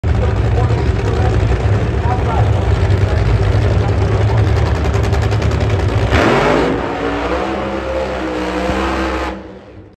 BOBBY COTTRELL | NOSTALGIA FUNNY CAR BURNOUT💨💨💨
NHRA💥Nostalgia Funny Car Burnout💥Bobby Cottrell💥Nevada Nationals💥Las Vegas Motor Speedway💥Saturday, October 29th 2022.